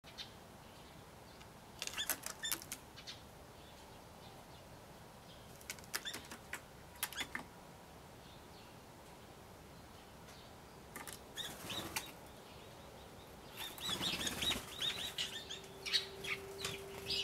かすかに鳴いてるヒヨドリ Bulbul chirping faintly
餌を没収されてもまだ餌台に来てるヒヨドリです。　そして微かに鳴いてます。
2013年2月20日 ivis

Bulbul still came to the bird restaurant even after its foods were taken away.  And it was calling faintly.